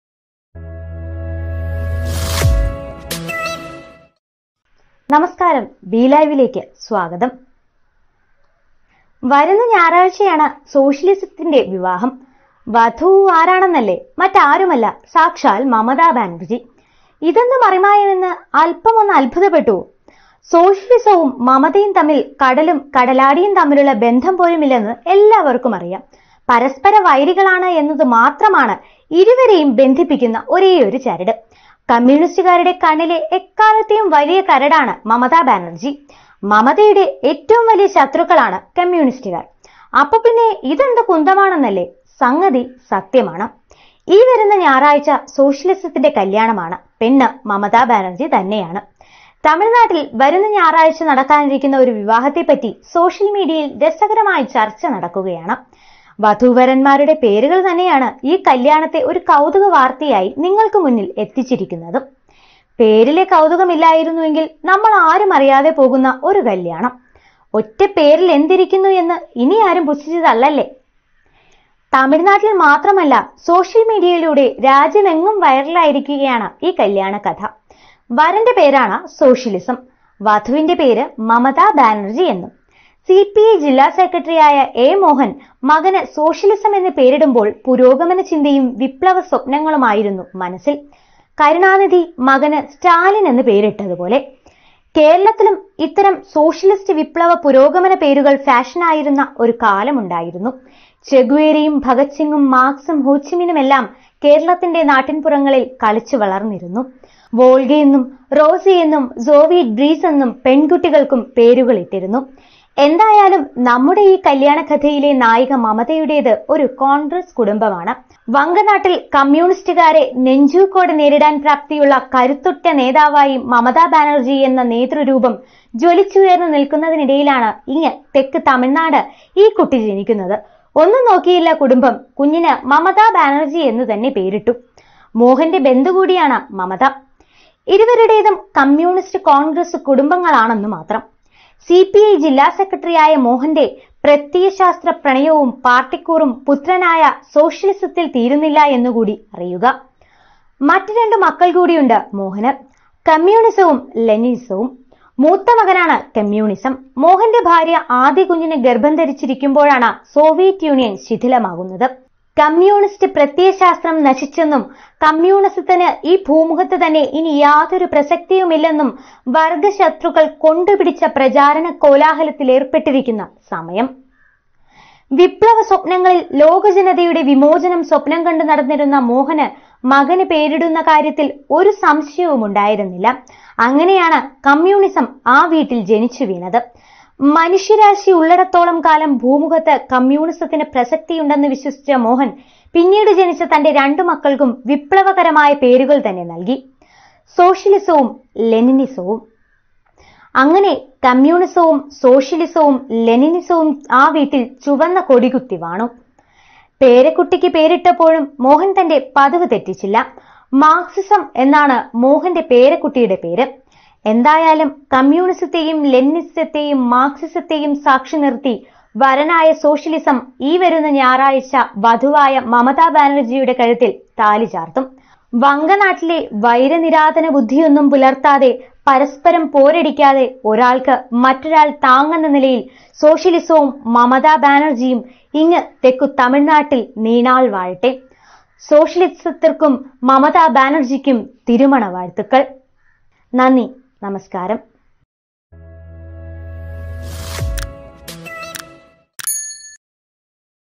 BLive Videos | സോഷ്യലിസത്തിൻ്റെ വിവാഹം ഞായറാഴ്ച, വധു മമത ബാനർജി!
വരുന്ന ഞായറാഴ്ചയാണ് സോഷ്യലിസത്തിൻ്റെ വിവാഹം. വധു ആരെന്നല്ലേ...
മറ്റാരുമല്ല സാക്ഷാൽ മമത ബാനർജി!

ഇതെന്തു മറിമായം എന്ന് അല്പമൊന്ന് അത്ഭുതപ്പെട്ടുവോ?

സോഷ്യലിസവും മമതയും തമ്മിൽ കടലും കടലാടിയും തമ്മിലുള്ള ബന്ധം പോലുമില്ലെന്ന് എല്ലാവർക്കും അറിയാം. പരസ്പര വൈരികളാണ് എന്നതു മാത്രമാണ് അവരെ ബന്ധിപ്പിക്കുന്ന ഒരേയൊരു ചരട്.

കമ്മ്യൂണിസ്റ്റുകാരുടെ കണ്ണിലെ എക്കാലത്തേയും വലിയ കരടാണ് മമത ബാനർജി. മമതയുടെ ഏറ്റവും വലിയ ശത്രുക്കളാണ് കമ്മ്യൂണിസ്റ്റുകാർ.